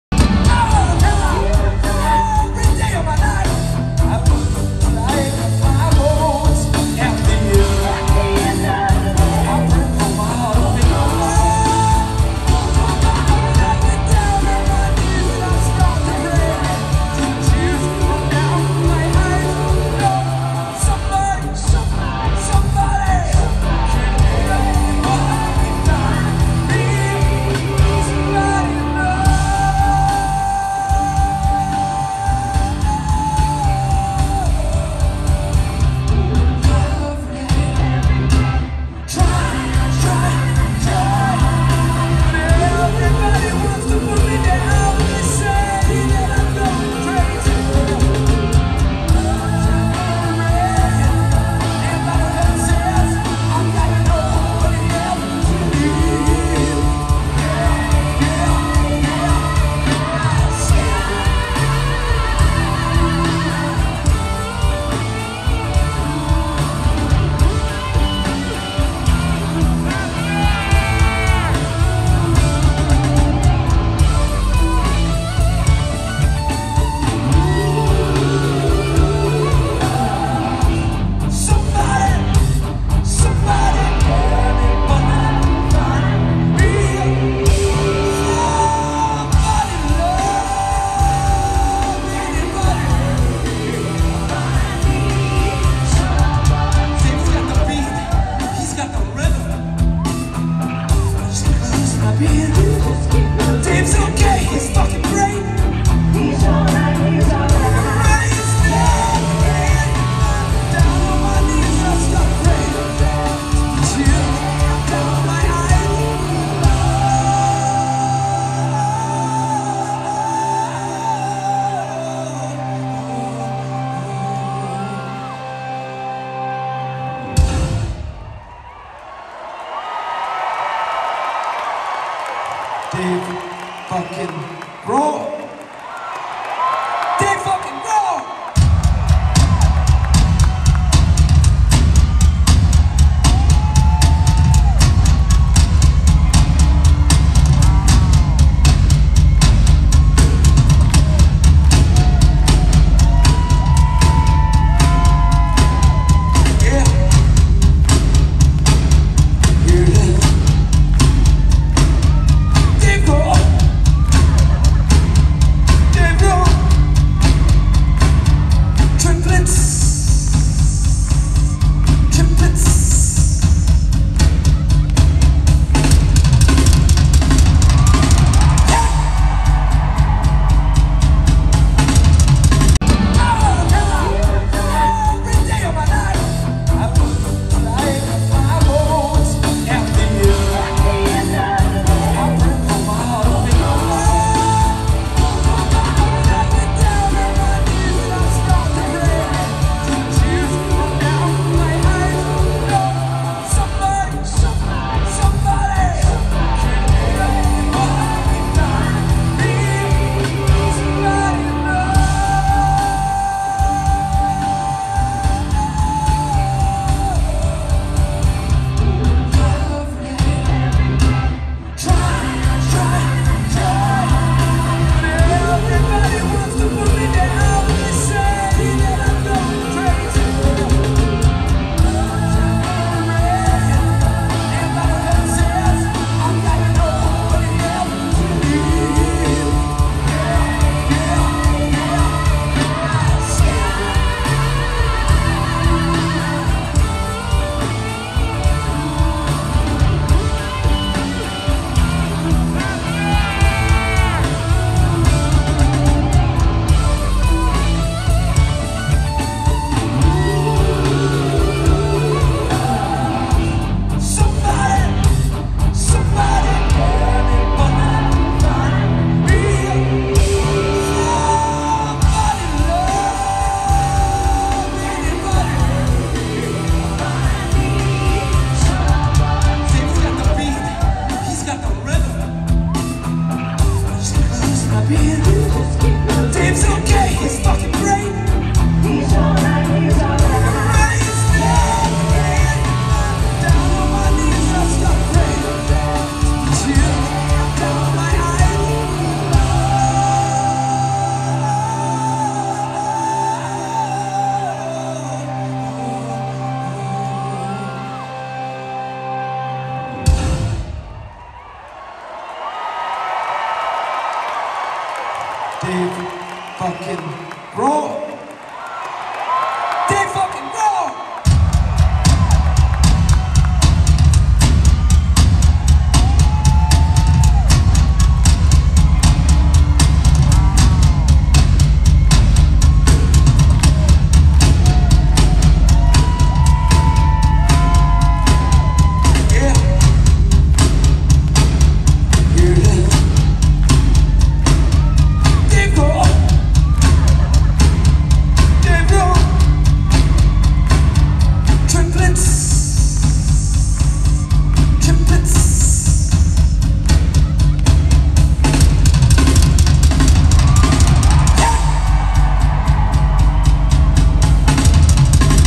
Foo Fighters 12/4/21 Vegas: Taylor Sings! part 1
Taylor steps out from the kit to call up his inner Freddie Mercury, and to give ol' Dave a bit of a workout on the skins. RIP Taylor . . .